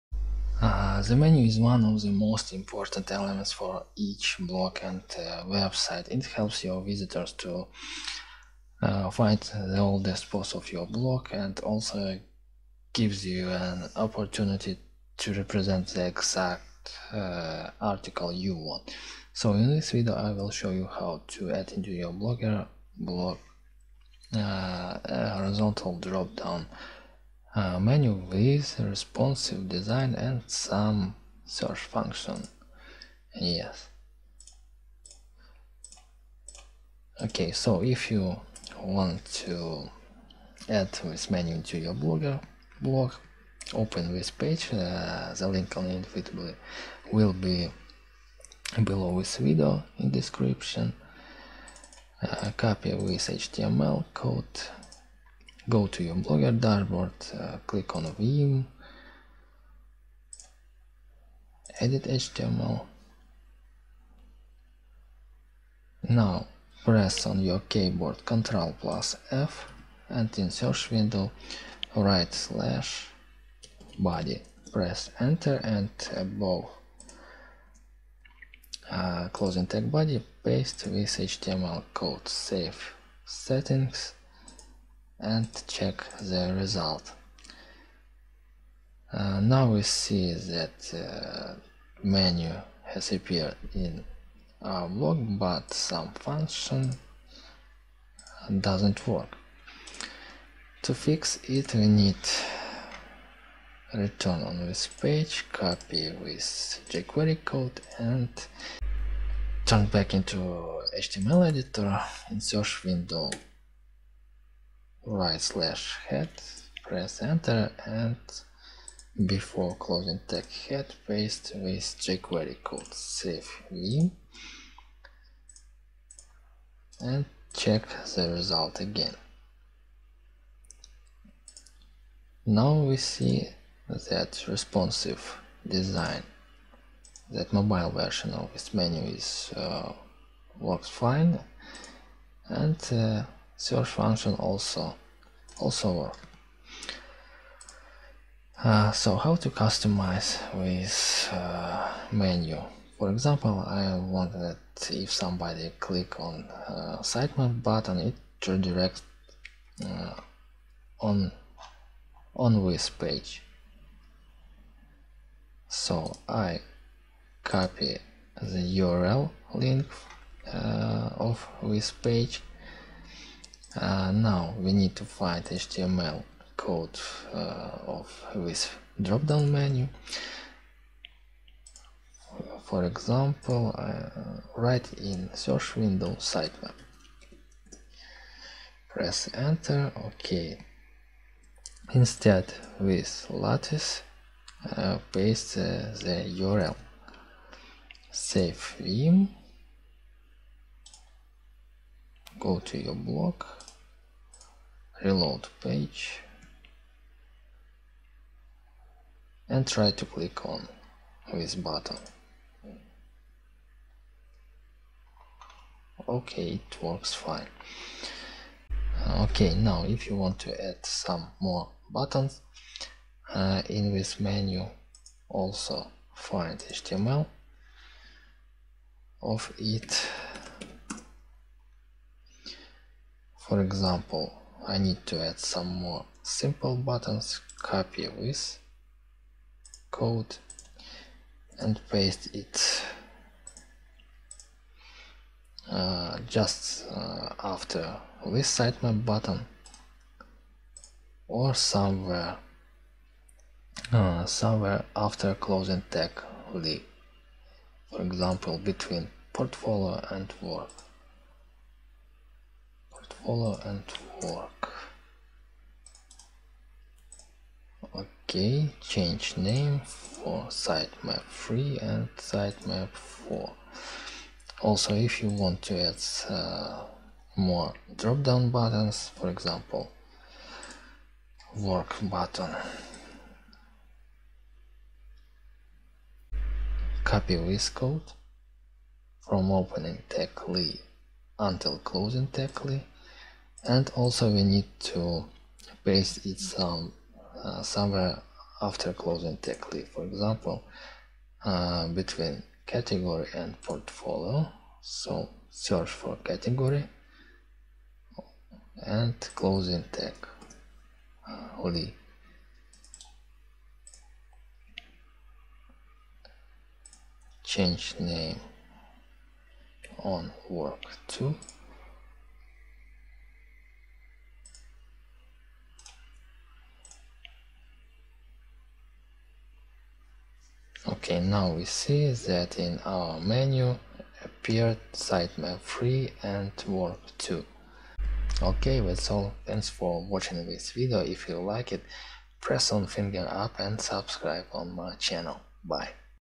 How to Add Dropdown Menu in Blogger (2025)
The menu is one of the most important elements for each website and blog. It helps your visitors to find the oldest posts, also it gives you a chance to represent the exact article you want. Search engines also like when the site has navigation elements because it greatly simplifies the movement on it.